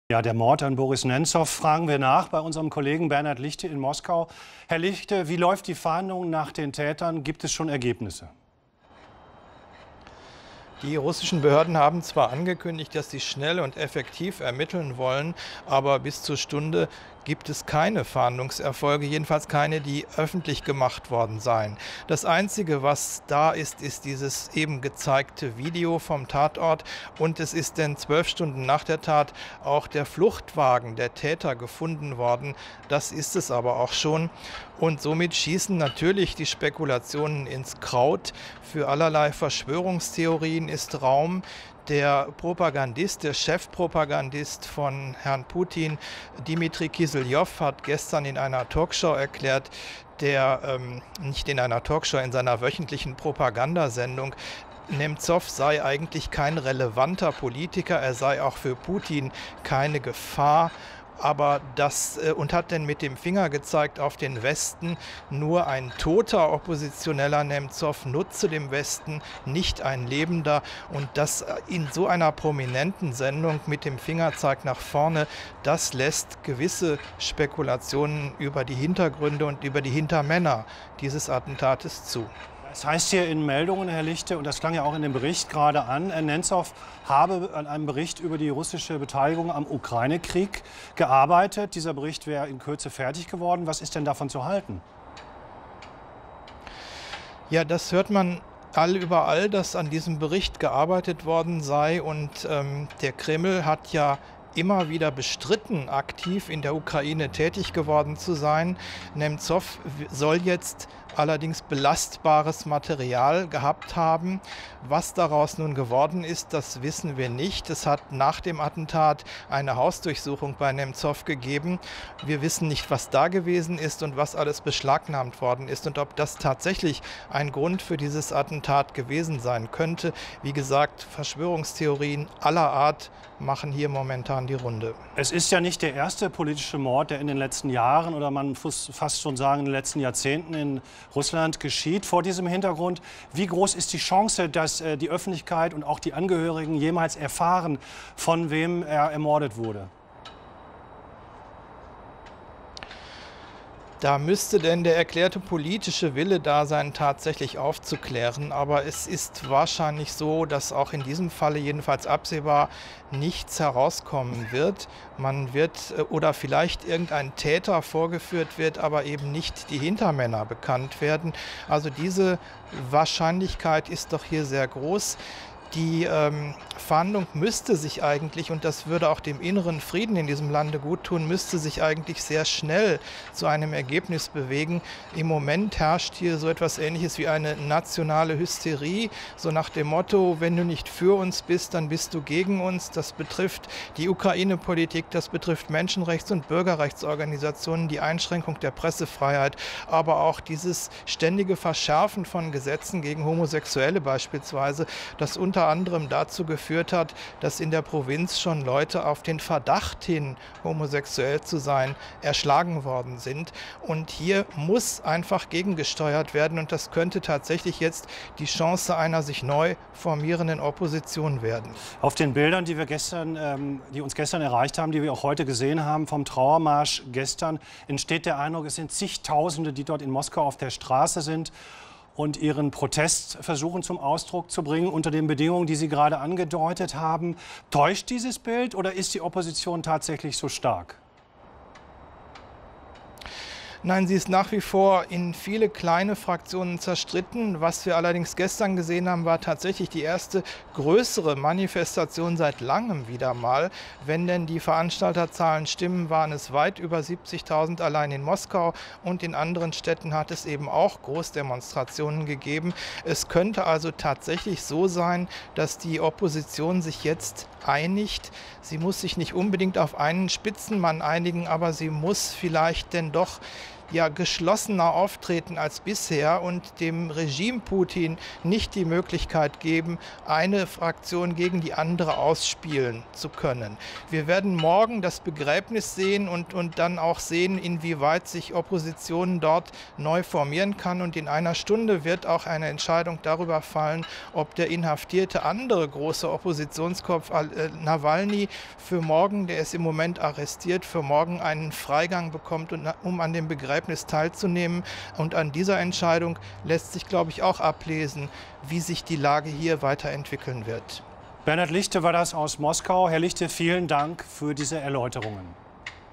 Mord an Boris Nemzow: Bernhard Lichte mit aktuellen Informationen am 02.03.2015
Hans-Werner Fittkau im Gespräch mit Bernhard Lichte in Moskau  über den Mord an dem Oppositionspolitiker Boris Nemzow und den Trauerkundgebungen in Russland